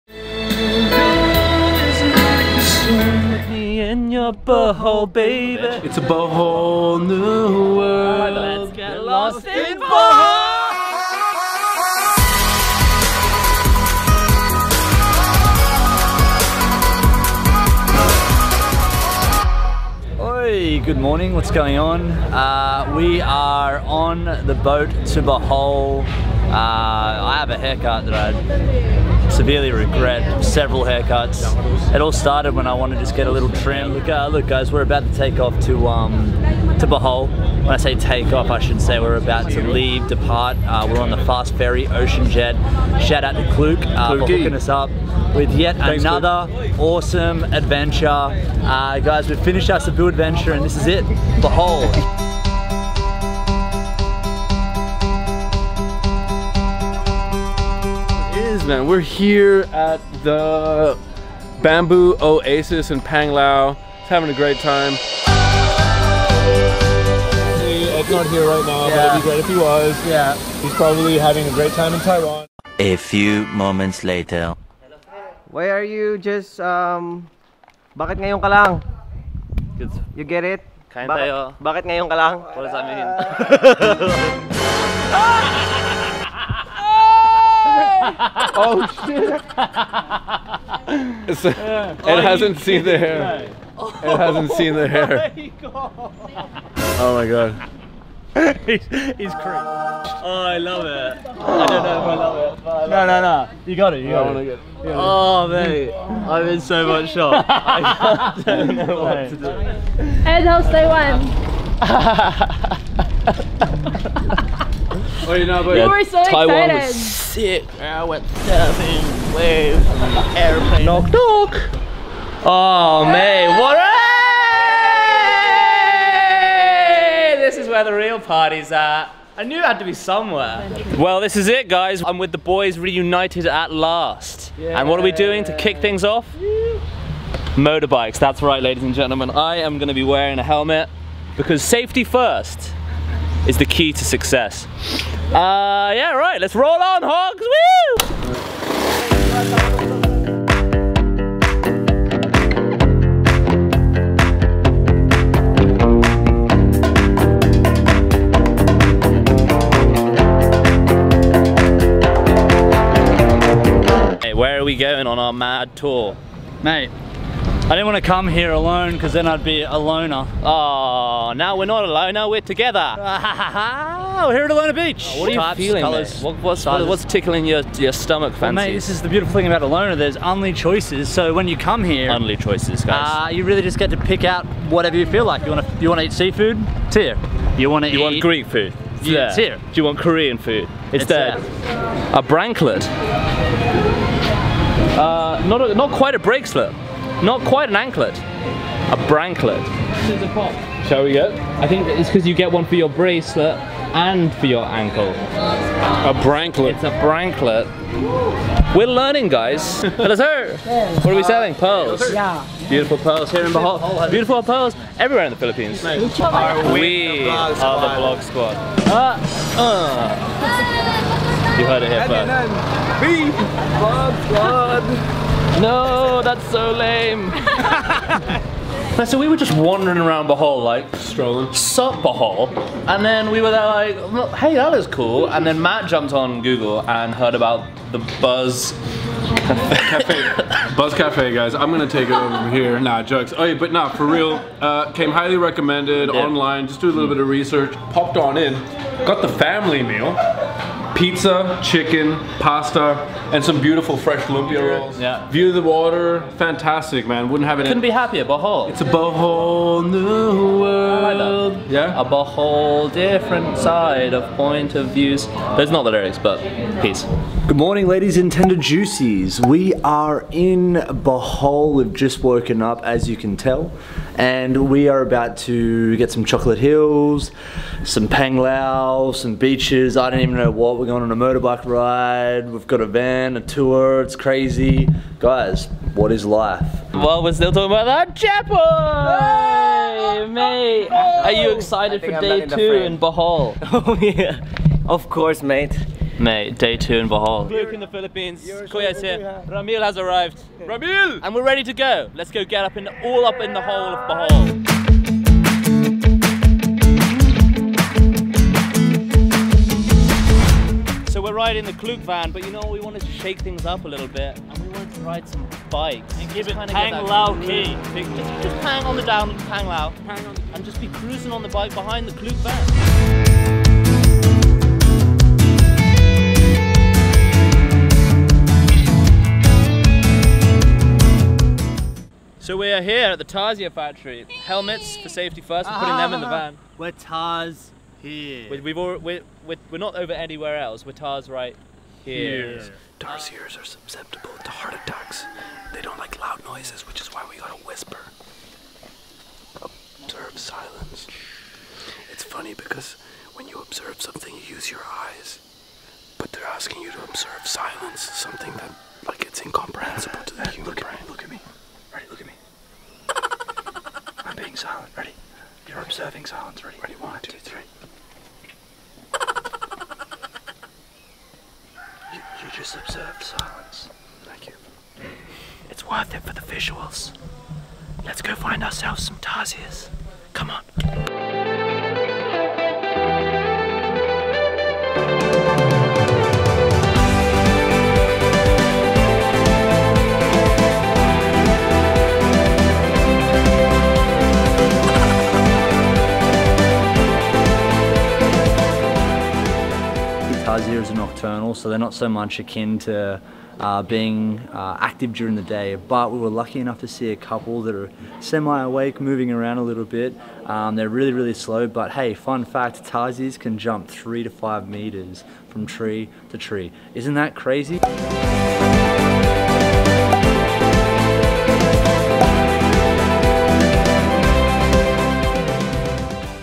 Does Ed Love Ben's New Haircut? | Bohol | Lost In The Philippines
After our crazy adventure in Cebu, Ed finally joined us in Bohol.

We traveled Bohol in motorbikes as it is one of the best ways to explore a region. Find out the places we've been and the people we met!

Shoutout to ShopBack and Klook for the amazing travel deals!

If you want to join us on our next adventure be sure to check out our Facebook page for all the latest competition updates: